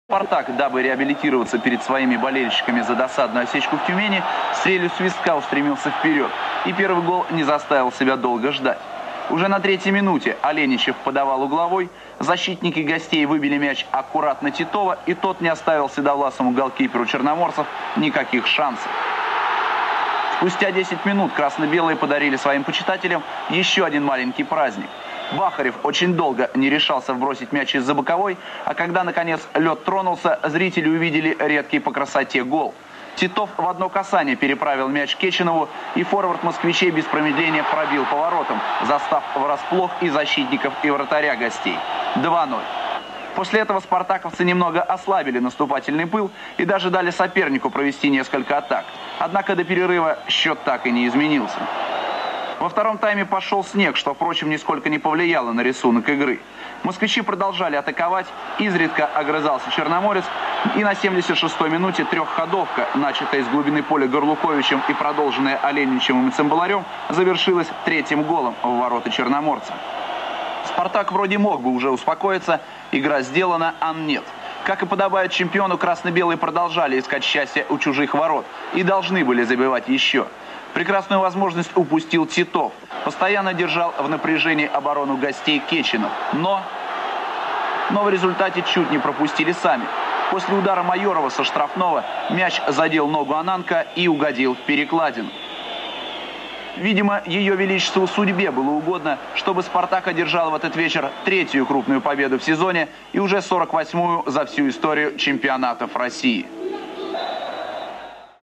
СПАРТАК - Черноморец (Новороссийск, Россия) 3:0, Чемпионат России - 1997
12 апреля 1997 года, Чемпионат России - 1997,
Москва, Стадион "Локомотив", 7000 зрителей
Голы: Егор ТИТОВ (3), Валерий КЕЧИНОВ (11), Илья ЦЫМБАЛАРЬ (75)